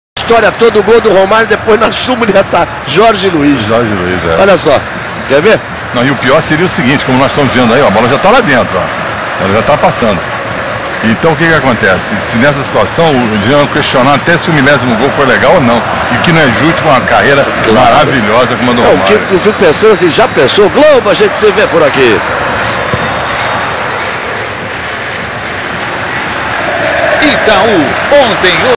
Romario e seu gol quase mil.
Romario e mais uma tentativa frsutrada de fazer seu gol 1000